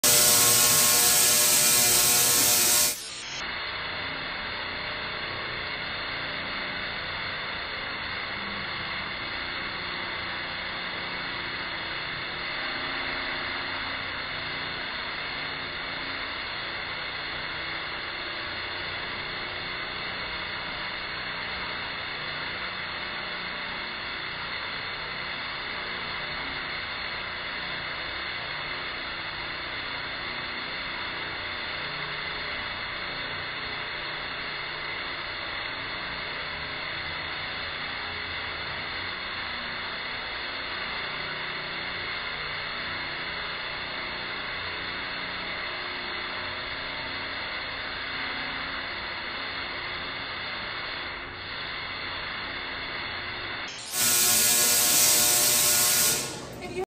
Tesla coil
Tesla coil display at the LA Griffith observatory